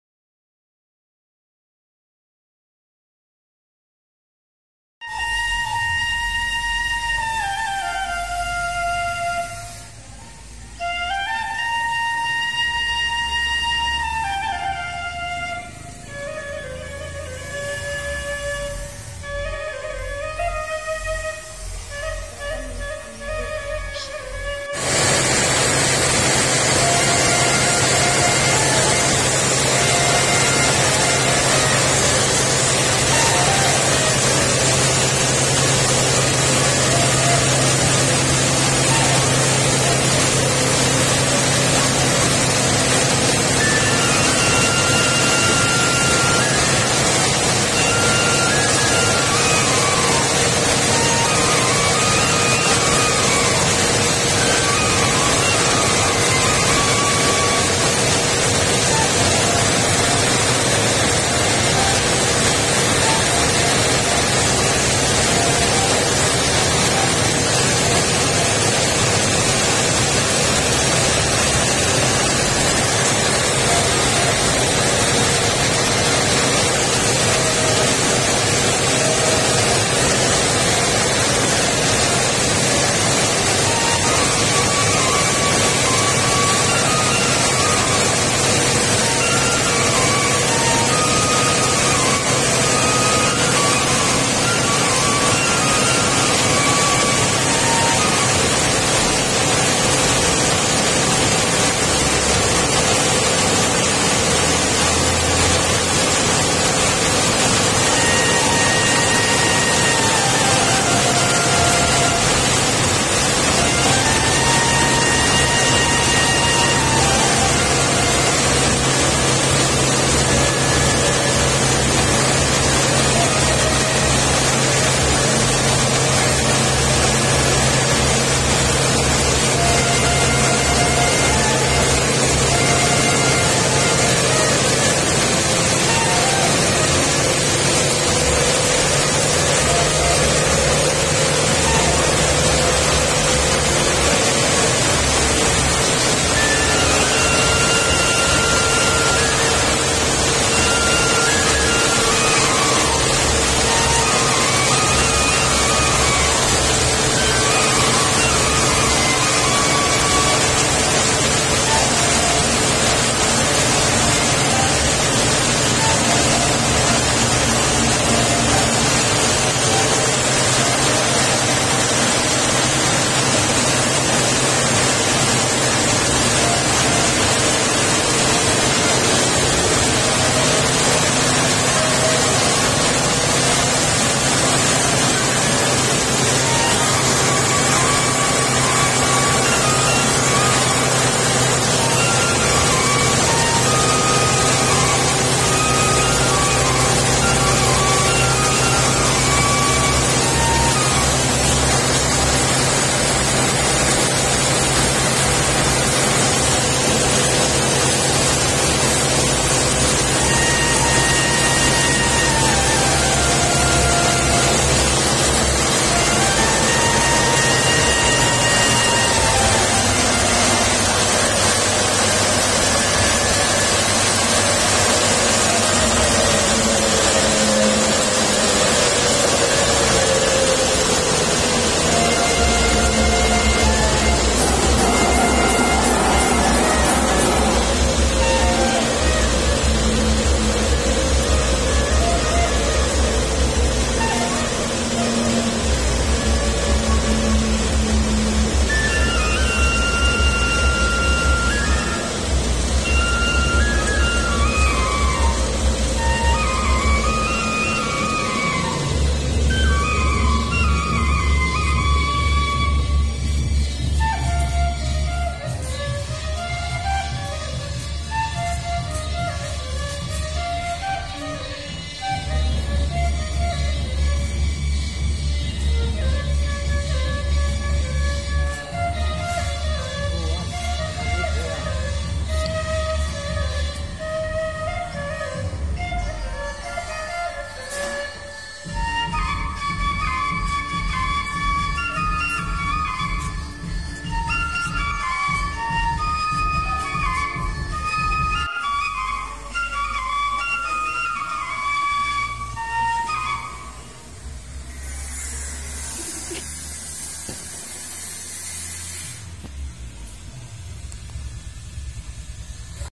KOPI GAYO /PROSES PENGGILINGAN / SUARA SULING DAN MESIN
Proses penggilingan kopi